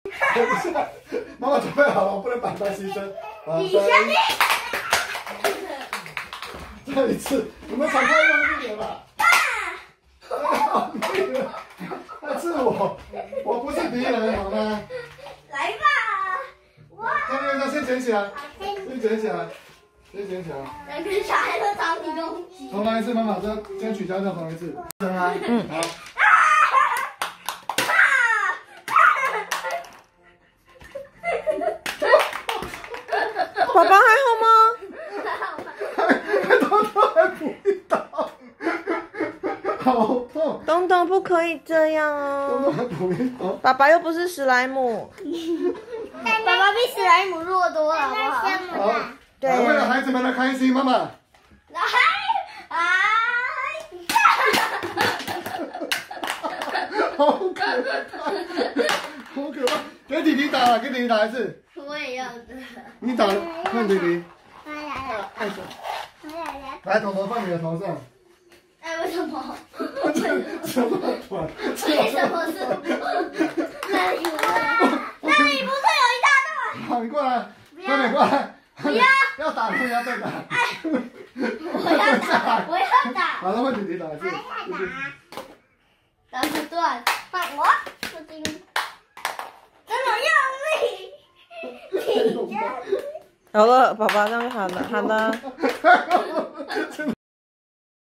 打鬧日常，哥哥揮劍弟弟補刀，爸爸遭殃了｜咚2y1m12d 桐8y10m
打鬧日常，哥哥和弟弟一起揮劍，爸爸遭殃了..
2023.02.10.
咚2y1m12d，桐8y10m12d

拍攝工具:
Samsung S10+
GoPro Hero 8
SONY RX100M5A
剪輯工具:
iMovie
final cut pro